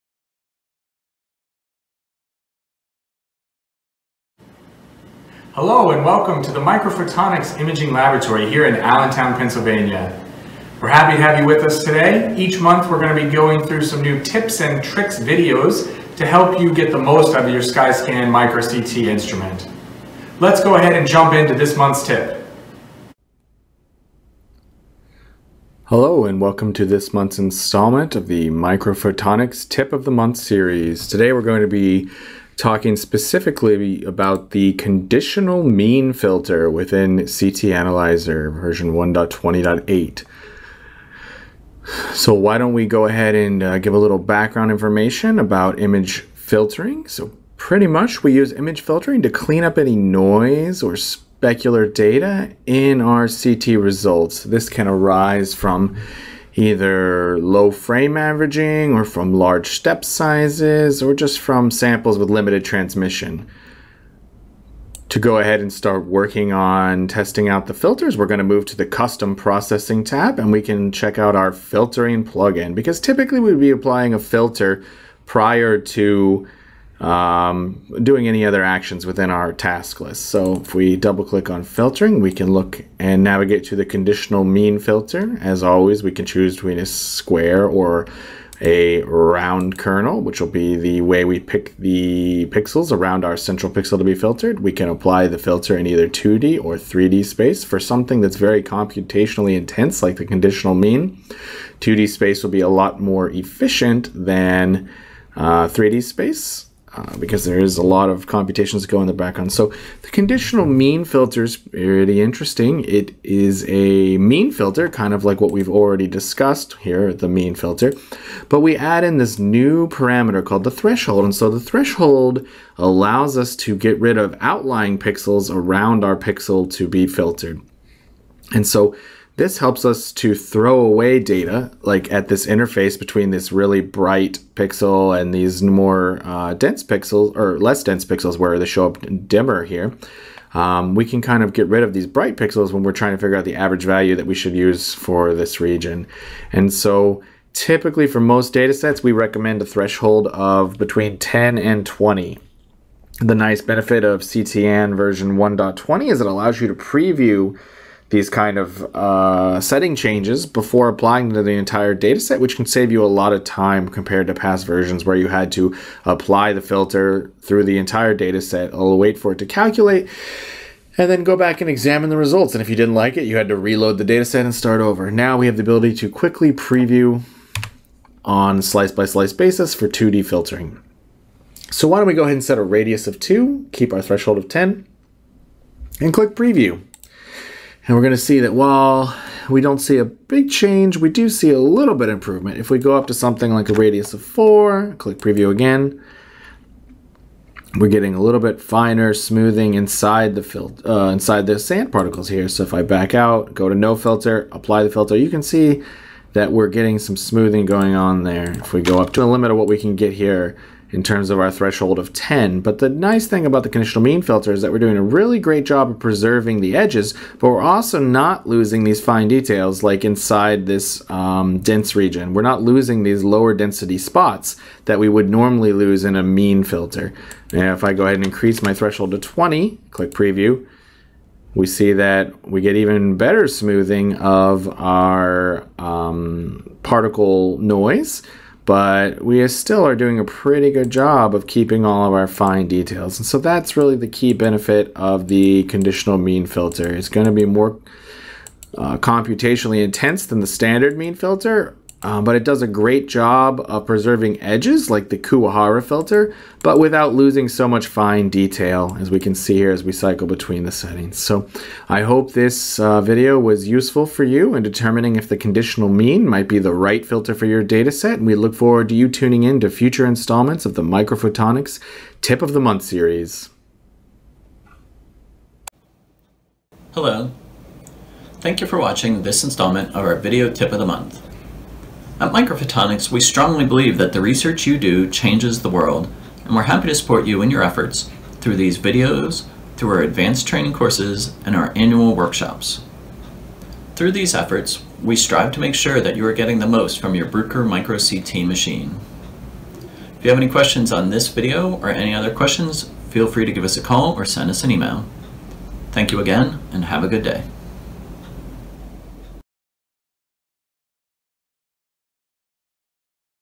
Tip of the Month - CTAn Image Filtering - Conditional Mean Filter
This instructional video provides information and best practices for utilizing the Conditional Mean filter from CTAn with your SkyScan datasets.